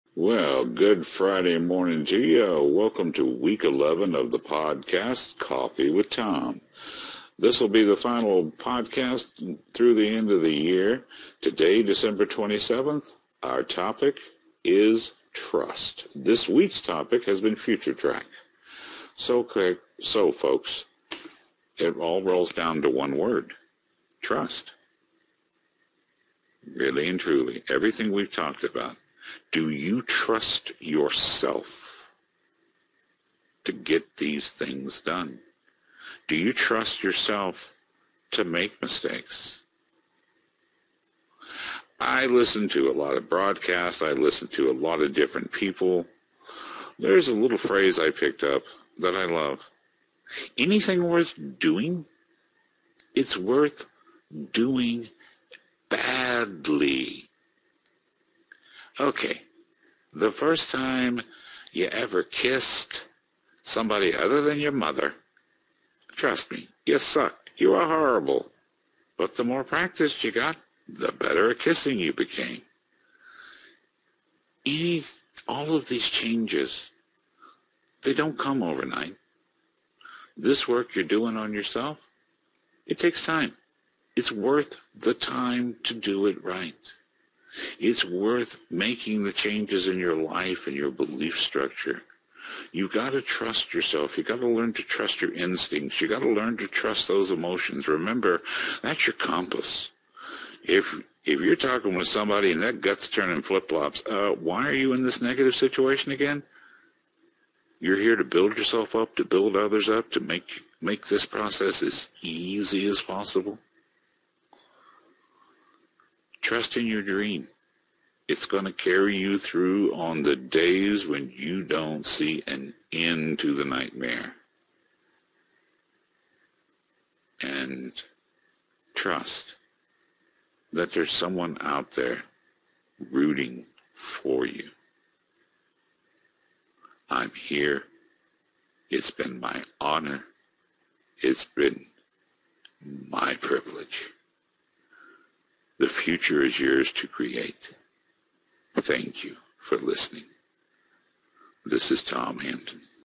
Coffee with Tom: Trust
Welcome to the final Coffee with Tom podcast! Anything worth doing, is worth doing badly. The more practice you get, the better you get. Changes don’t come overnight, and its worth the time to learn to do it right. Let your gut be your compass! Want special invitation-only access to more podcasts from Tom?